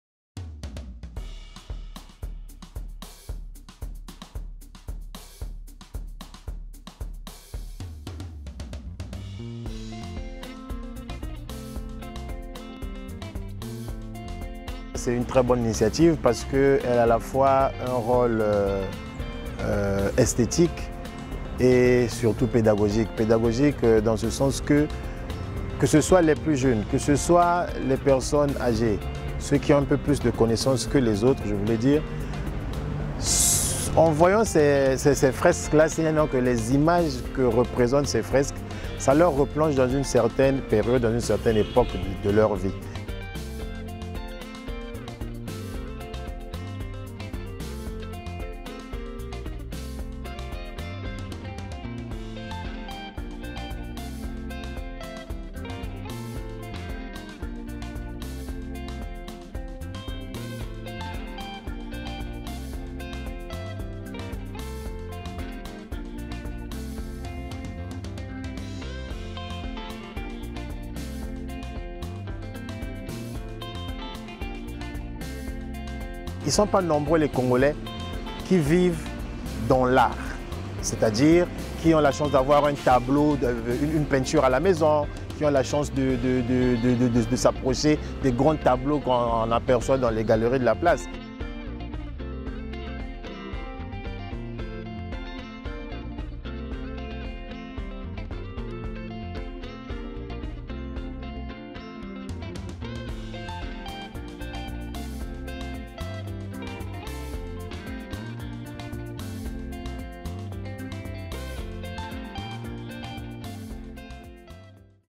Congo: des fresques rappellent l’histoire féconde du pays
A Pointe-Noire, la capitale économique de la République du Congo, des fresques murales invitent à un voyage intérieur pour découvrir un pan d'histoire et illuminer les bâtisseurs d'une république vieille de 64 ans